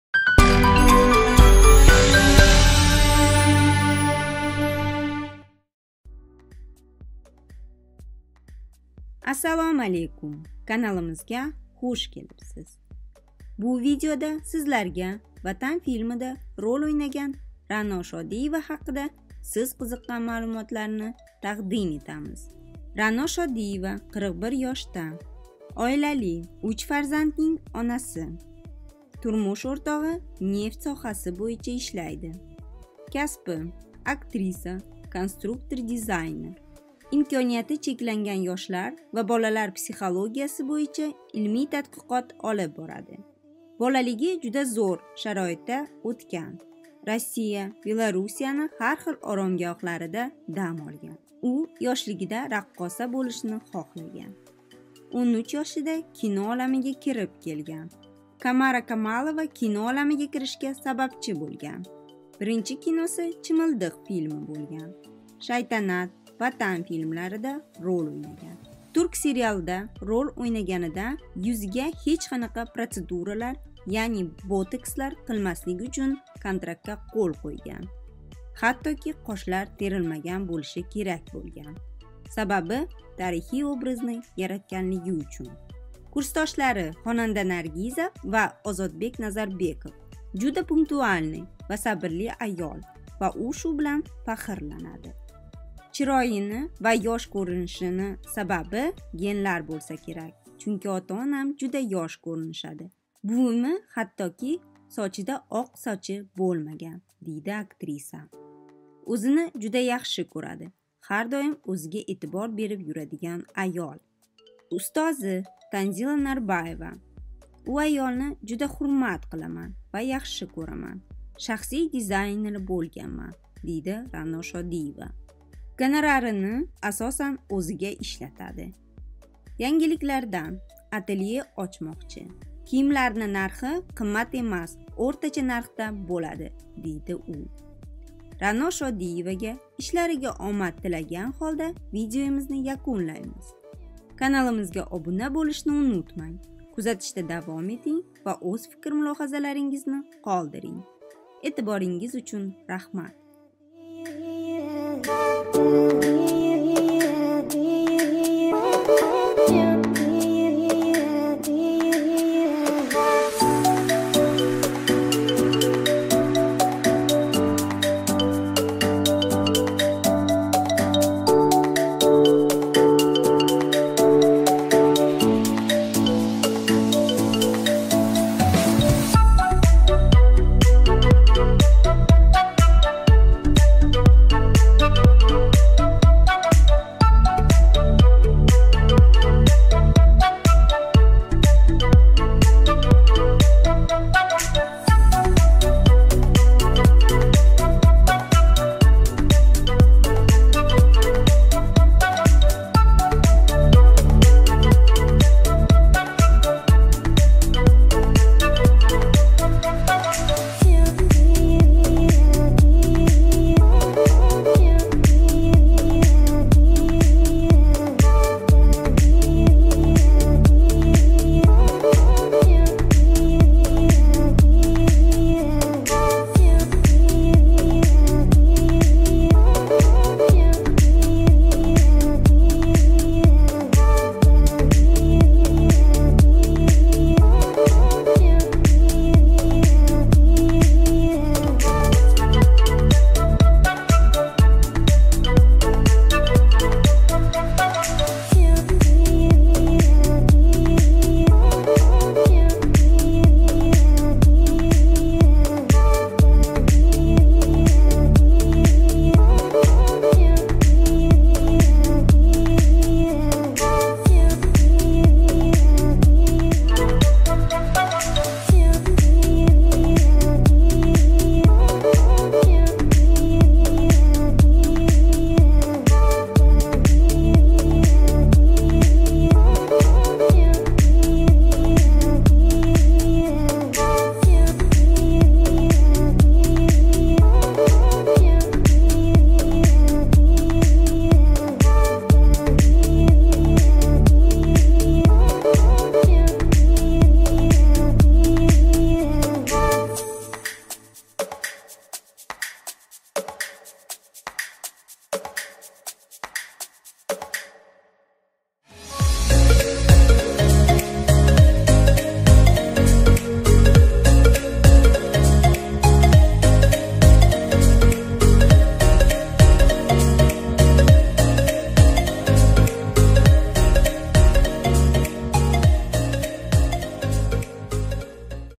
RANO SHODIYEVA HAQIDA, UNING YOSHLIGI, YOSH KO'RINISH SIRLARI, TANZILA NARBAYEVA UNGA KIM???
Salom barchaga! Bu kanalda barcha mashxur insonlarning eng sara foto suratlarini tomosha qilishingiz va ular haqida qiziqarli ma'lumotlarga ega bo'lishingiz mumkin.

Bu videoda JURNALIST serialidagi RANO SHODIYEVA haqida qisqacha ma'lumot va photo suratlari bilan tanishing.

Fotolar  yoqdimi ? Unda Instagram Olami kanaliga obuna bo'lishni va qo'ng'iroqni bosishni unutmang!

Etiboringiz uchun Katta Rahmat!!!